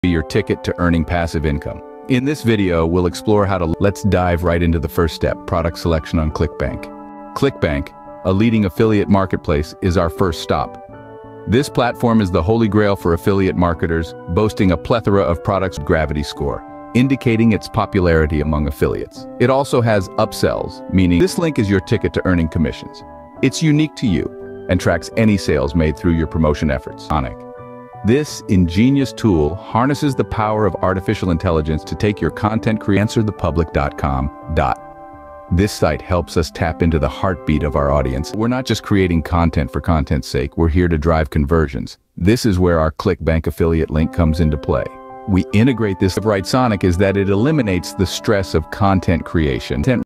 Ultimate Affiliate Marketing Strategy with Clickbank
In this video, we are unveiling the best Clickbank Affiliate Marketing Strategy using Writesonic, Vocal Media, and Pinterest. Whether you're just starting out or looking to enhance your current strategy, this proven method will help you boost your Clickbank affiliate sales.

Learn how to leverage Writesonic's AI-powered content creation, Vocal Media's publishing platform, and Pinterest's visual search engine to increase your affiliate commissions.

OUTLINE:

00:00:00 Introduction and The Power of Affiliate Marketing
00:00:20 Choosing the Right Product on Clickbank
00:01:12 Content Creation with Writesonic
00:02:40 Content Publishing on Vocal Media
00:03:33 Driving Traffic with Pinterest
00:04:30 Call to Action